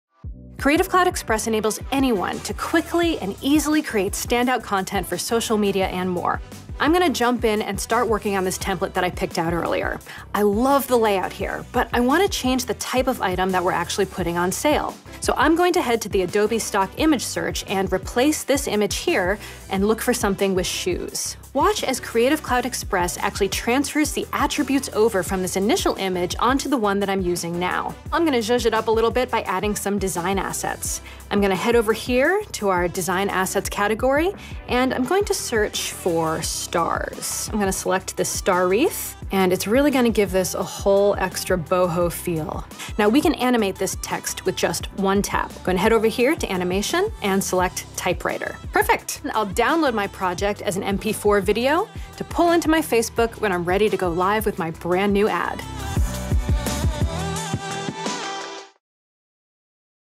Create An Ad - Creative Cloud Express Tutorial | Adobe Express
Learn how to edit an ad in this tutorial. Build design assets fast and animate text with just one tap. Add your own charm to templates on the web or on your phone. This is all easily achieved in Creative Cloud Express. First 14 days are FREE.